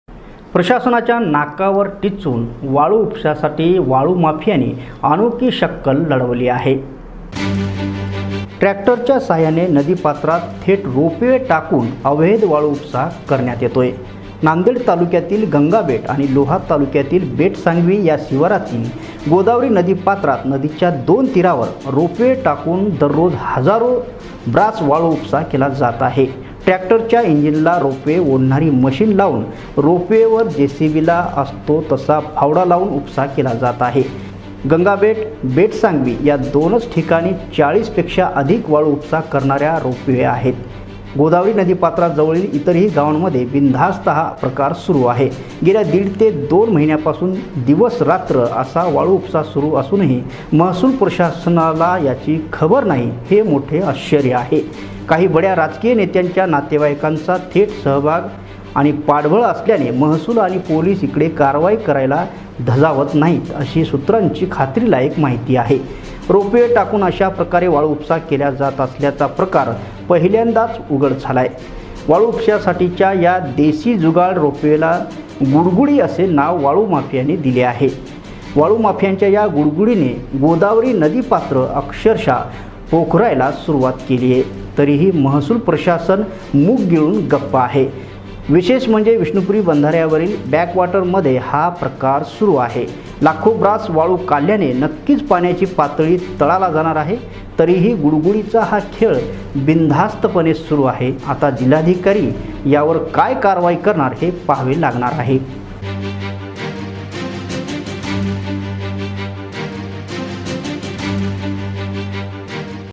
वाळू माफियांची अनोखी शक्कल- नदीपात्रात चक्क रोप वे टाकून वाळू उपसा - प्रशासन झोपेत
ट्रॅक्टरच्या इंजिनला रोप वे बांधून फावड्याच्या आधारे वाळू ऊपसा